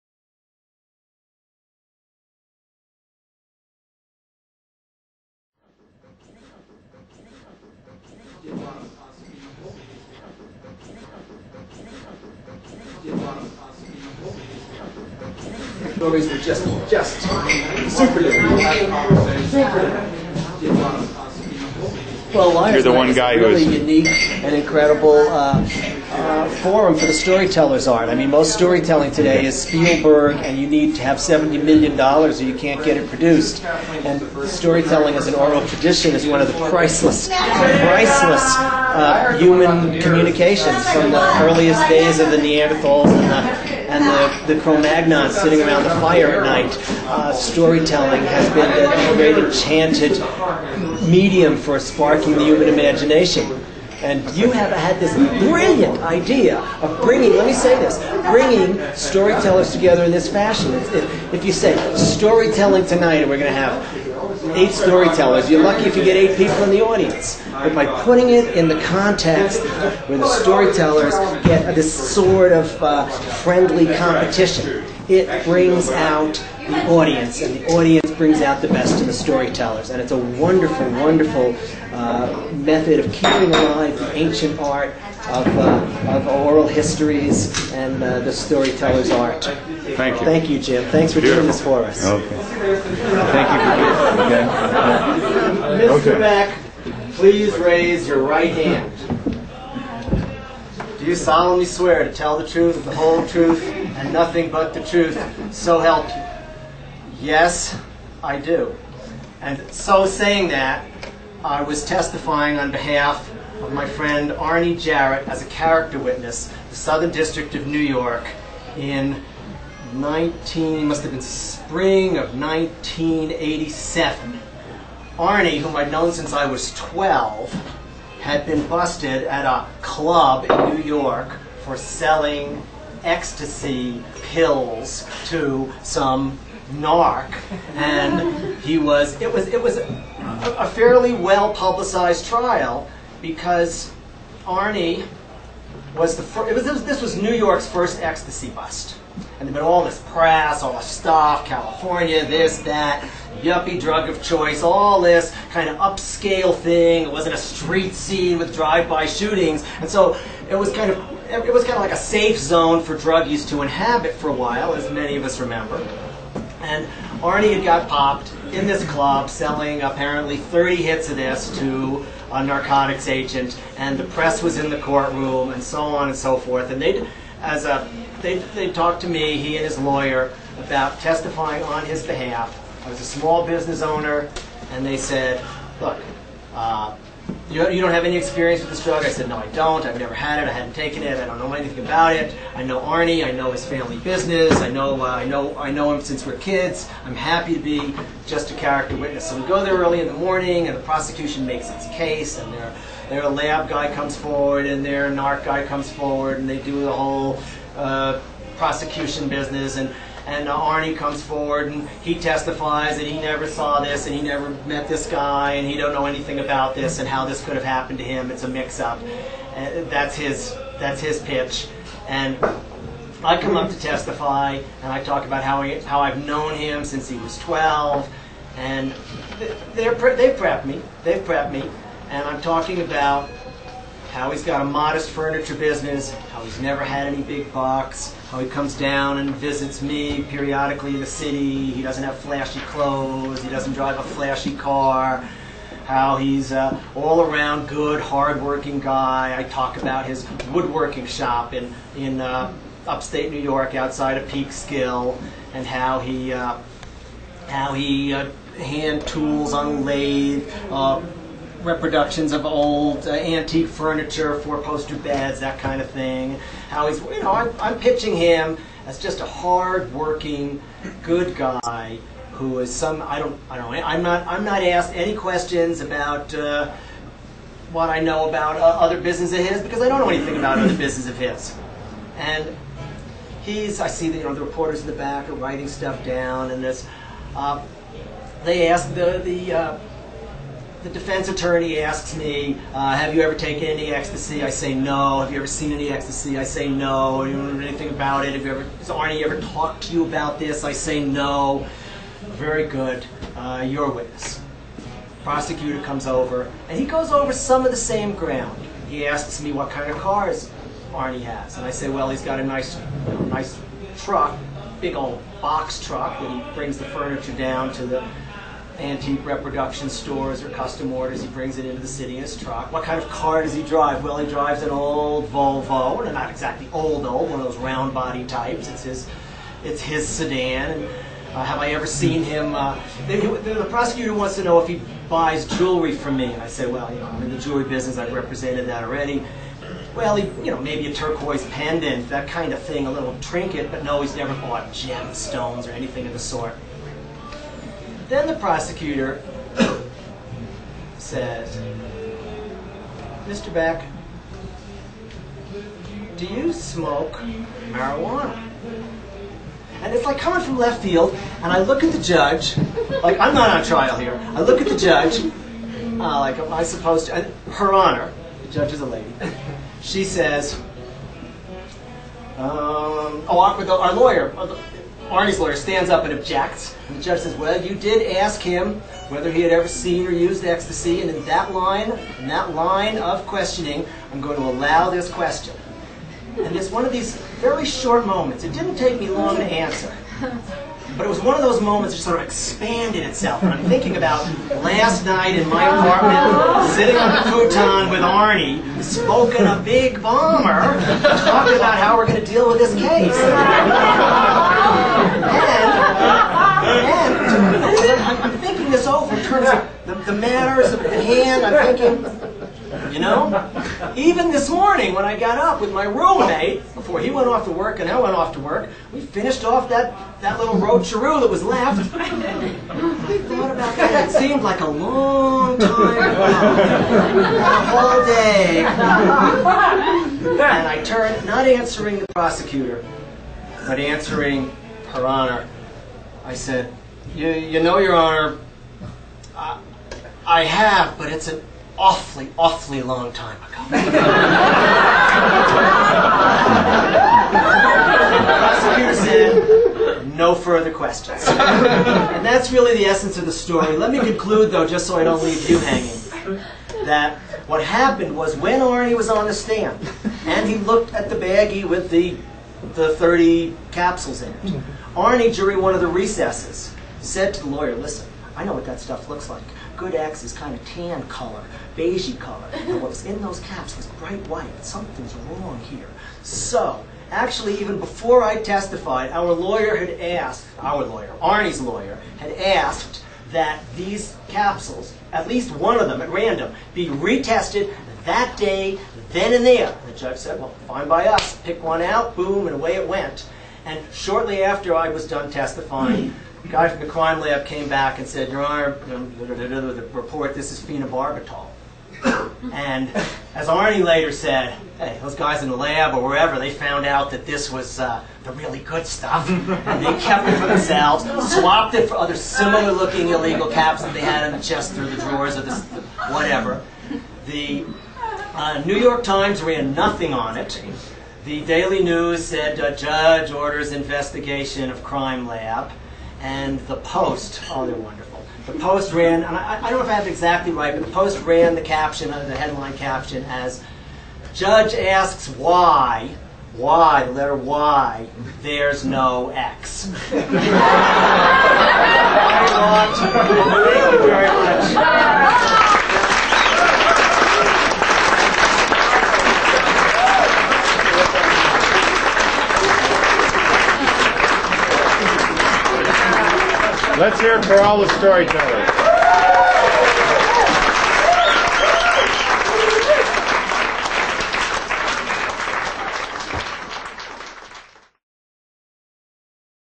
Liar's Night Grand Lie-Off -- Garrick Beck
From the 1997 Grand Championship Lie-Off climax of Santa Fe's long-running lying contest, a tale by Garrick Beck (which may or may not be true), hosted as always by Jim Terr at the Cowgirl Hall of Fame BBQ's Mustang room. To see all Jim Terr YouTube videos, enter HYMIEHYMIE in search line. for more glimpses of this strangely beautiful, beautifully strange town.